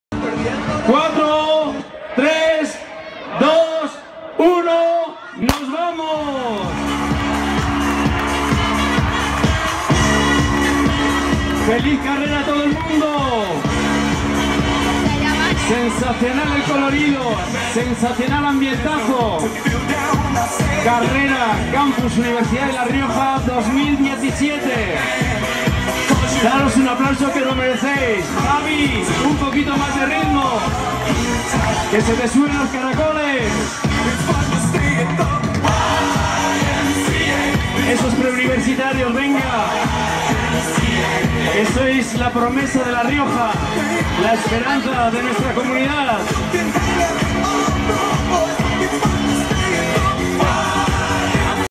Carrera Campus 2017
La Carrera Campus Universidad de La Rioja 2017 disputada el domingo 9 de abril ha contado con 1.100 participantes en sus distintas categorías. La prueba popular de 10.000 metros ha sido ganada por los corredores Nouriddine Benmeziane y Susana Arrúa en categoría masculina y femenina respectivamente.